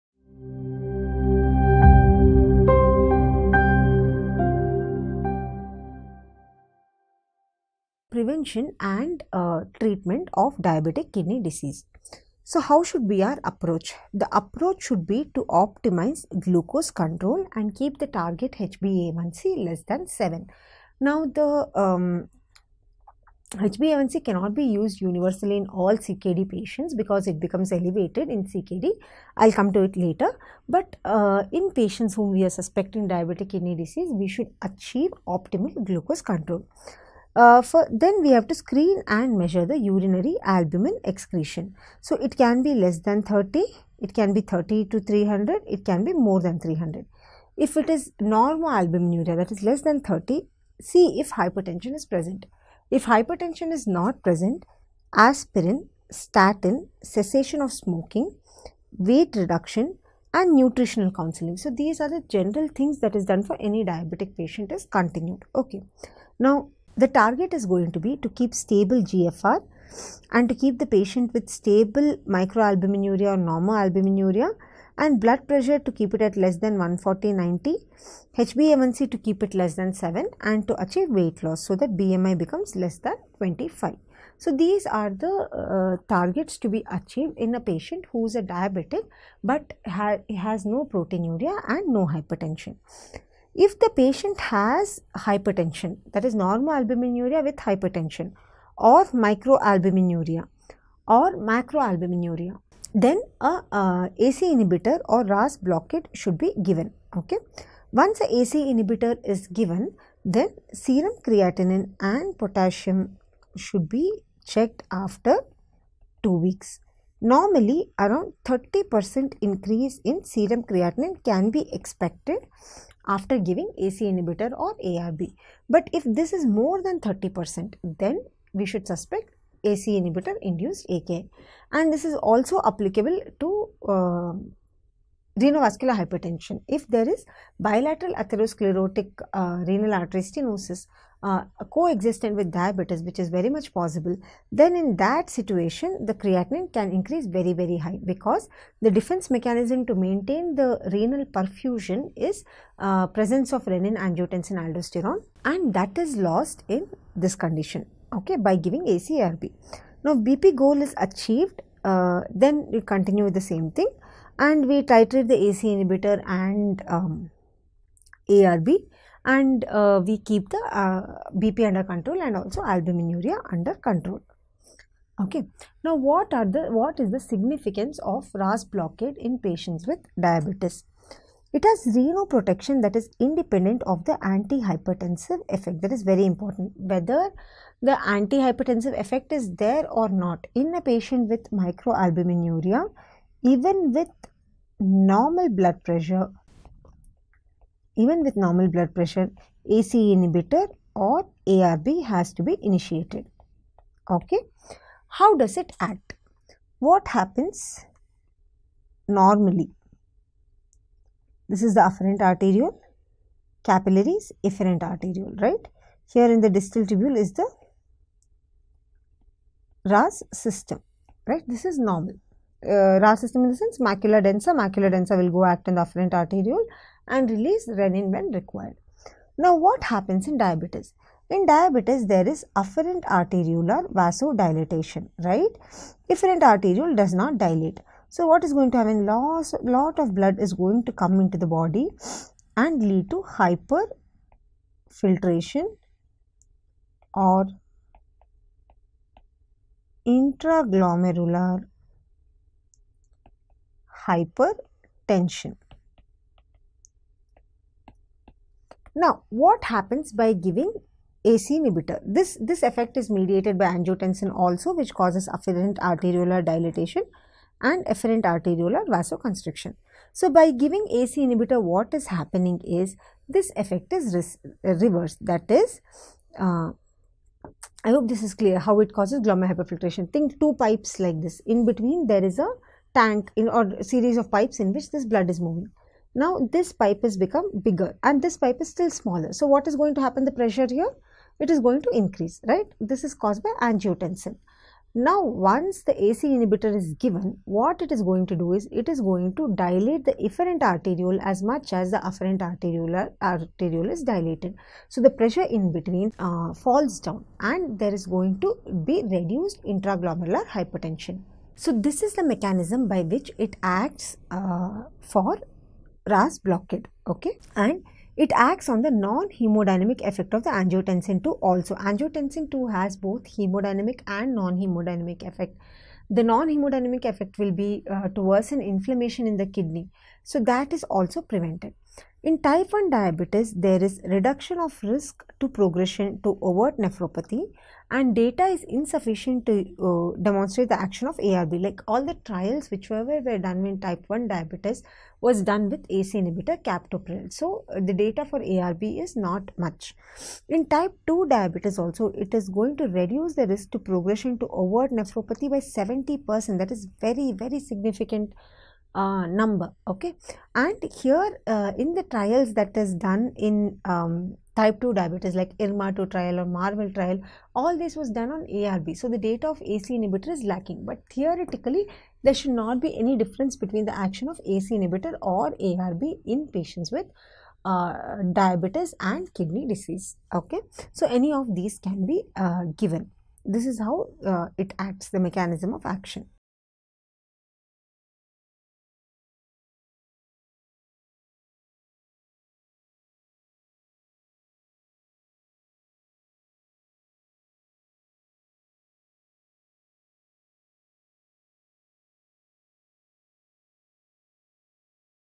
Prevention and Treatment of "DKD" | PrepLadder NEET SS
Watch Dr. Ganga RG's video on the Prevention and Treatment of DKD, a valuable resource for NEET Super Specialty in Medicine preparation. Gain insights into managing this condition effectively, equipping yourself with the knowledge needed for success in the exam.